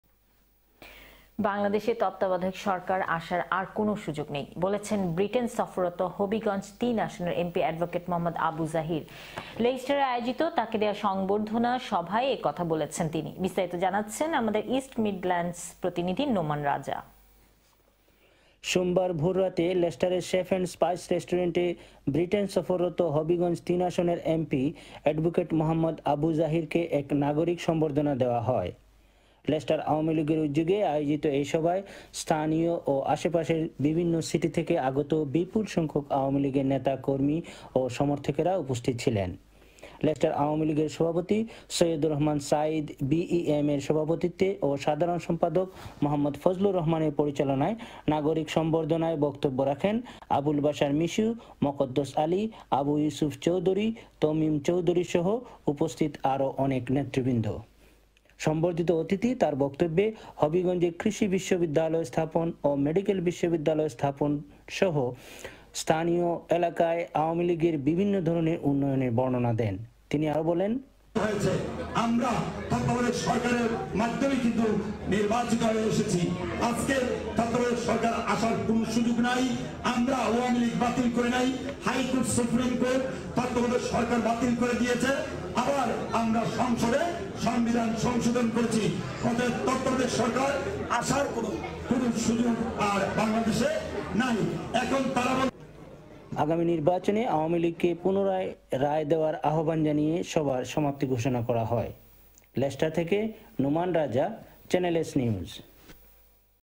Leicester Civic Reception of MP Advocate Md Abu Zahir
Channel S News (Sky 777) Leicester Awamileuge organised a civic reception for Visiting Bangladeshi  MP Advocate Md Abu Zahir on 31st July 2023.